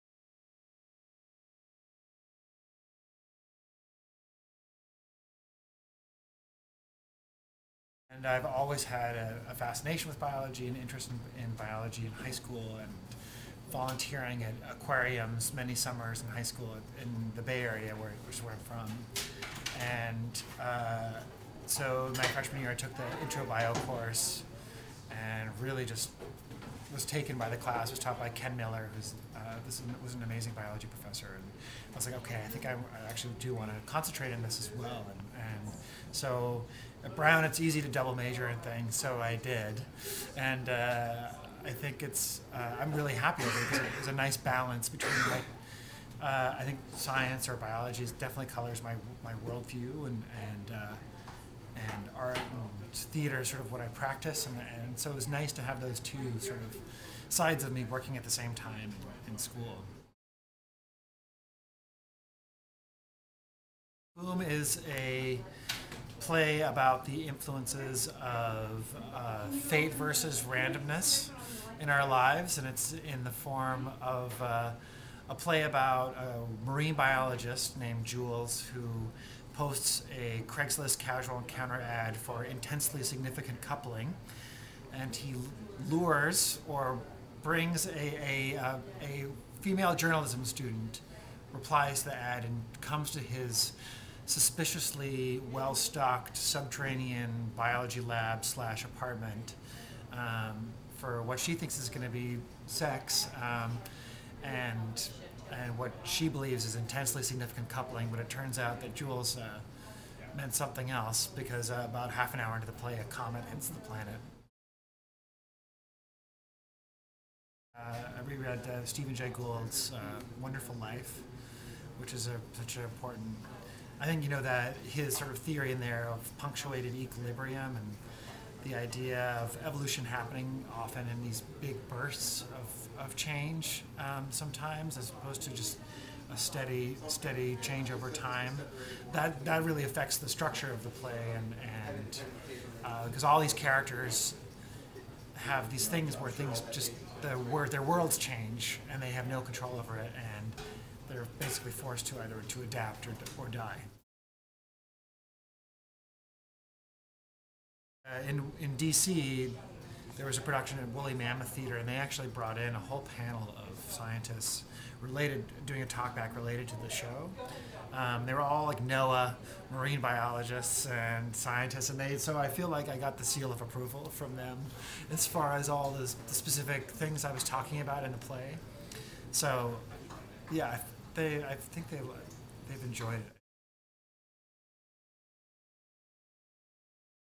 "boom" boosts science theatre
The Scientist's interview with playwright Peter Sinn Nachtrieb about his evolutionary biology-focused play, "boom."